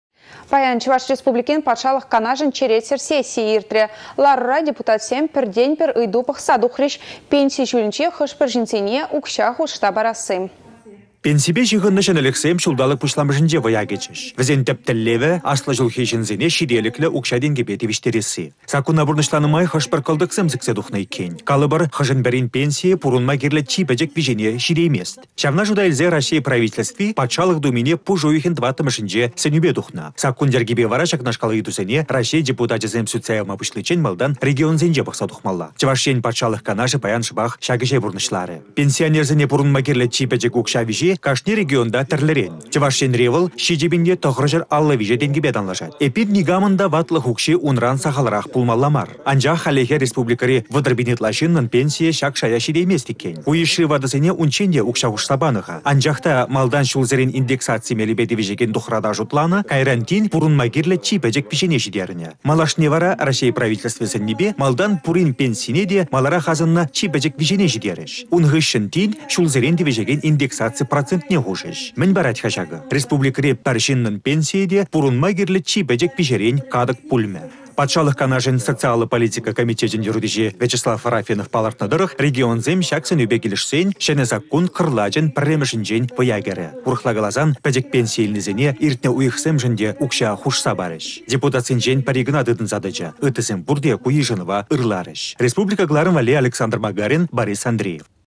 Аслă çулхи çынсене хушма пулăшу парĕç
Чăваш Республикин Патшалăх Канашĕн черетсĕр сессийĕ иртрĕ. Ларура депутатсем пĕртен-пĕр ыйту пăхса тухрĕç: пенси çулĕнчи хăшпĕр çынсене укçа хушса парасси.